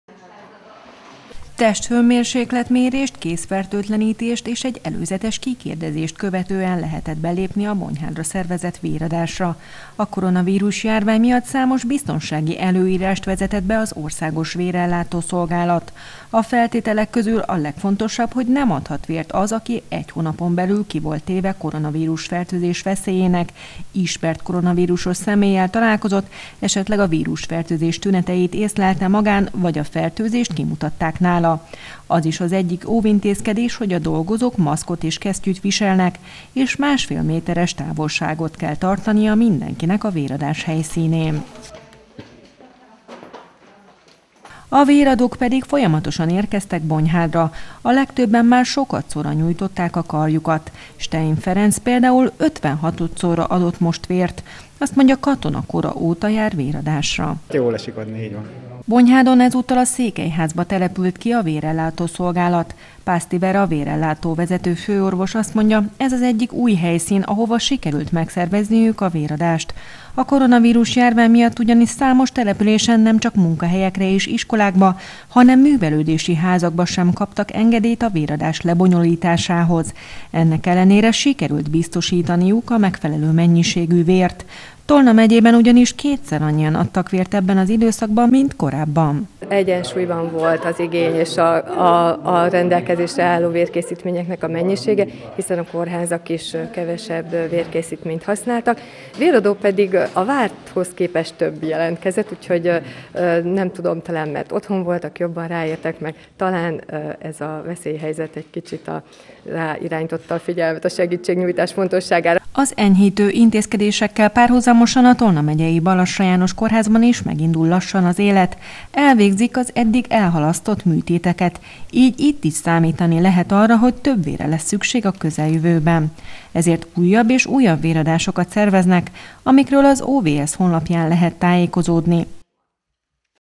Közel kétszer annyian adtak vért a koronavírusjárvány idején Tolna megyében. 20200602
Közel kétszer annyian adtak vért a koronavírusjárvány idején Tolna megyében, mint az azt megelőző időszakban, így nem okozott fennakadást a vérellátásban, hogy a munkahelyekre, iskolákba, és sokszor még a művelődési házakba sem lehetett szervezni váradást. A véradó szolgálat és a Magyar Vöröskereszt Tolna megyei szervezete új helyszíneket keresett, a véradók pedig jöttek és segítettek. Legutóbb Bonyhádon, a Székely Házban várták a véradókat.